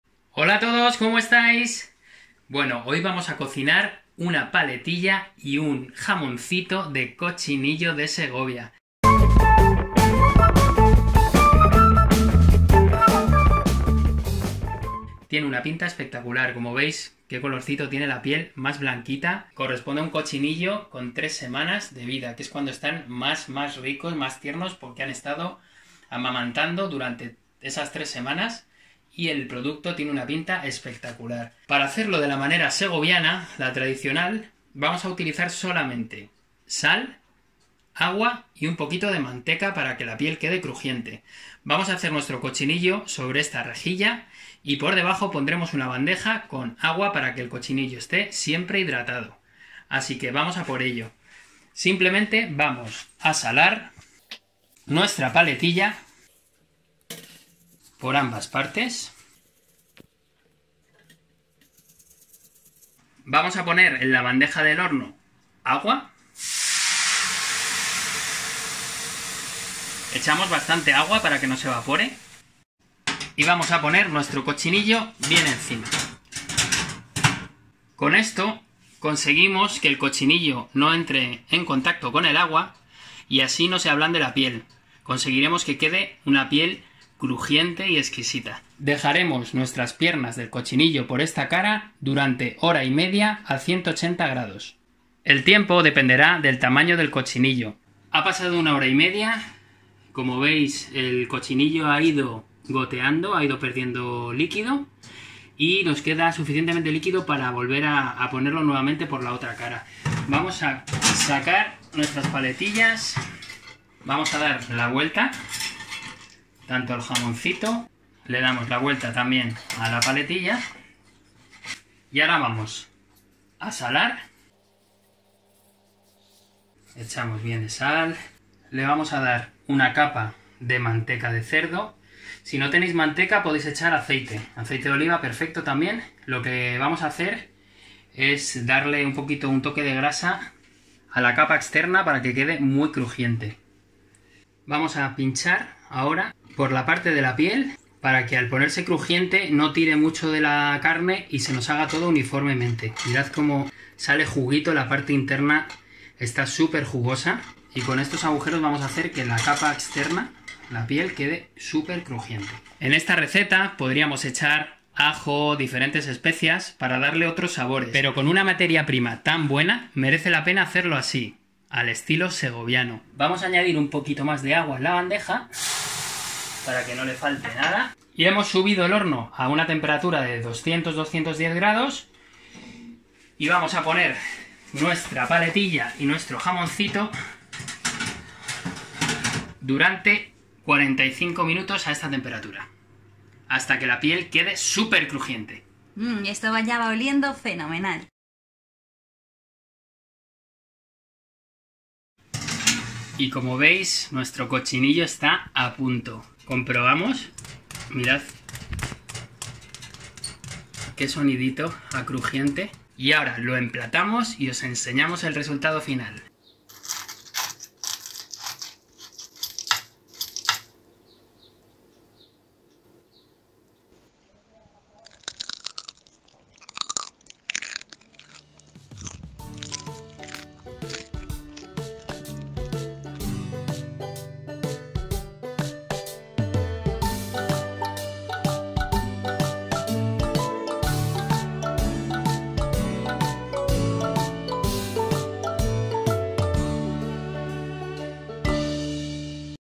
PALETILLA DE COCHINILLO súper CRUJIENTE | Crunchy toasted pork
Prepara fácilmente una PALETILLA DE COCHINILLO súper CRUJIENTE.

Veréis que con tan solo 5 ingredientes se consigue un platazo fácil y rápido. Cochinillo crujiente sobre bastones de patatas.

Con este producto tan de las dos Castillas haremos también una ensaladita que le irá de maravilla  ¡Manos a la cocina!

Paletilla de cochinillo crujiente

PALETILLA DE COCHINILLO súper CRUJIENTE
INGREDIENTES

1 Paletilla de cochinillo
1 Jamoncito de cochinillo
Sal, manteca y agua

PREPARACIÓN
Echar sal y manteca sobre las dos piezas.
Añadir agua abundante en la bandeja del horno para que esté hidratado.

Lo metemos 90 minutos a 180º
Lo sacamos del horno, aplicamos manteca y sal de nuevo.
Pinchamos con un palillo toda la piel para conseguir que quede crujiente y lo metemos de nuevo en el horno a 210º durante 45 minutos.

Lo podemos acompañar con ensalada o patatas como guarnicion.